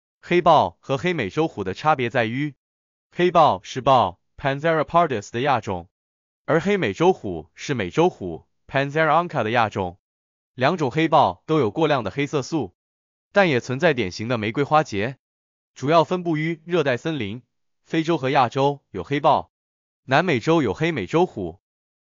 黑豹和黑美洲虎有什麼差別？
黑豹和黑美洲虎的差別在於，黑豹是豹（Panthera pardus）的亞種，而黑美洲虎是美洲虎（Panthera onca）的亞種。兩種黑豹都有過量的黑色素，但也存在典型的玫瑰花結。主要分佈於熱帶森林，非洲和亞洲有黑豹，南美洲有黑美洲虎。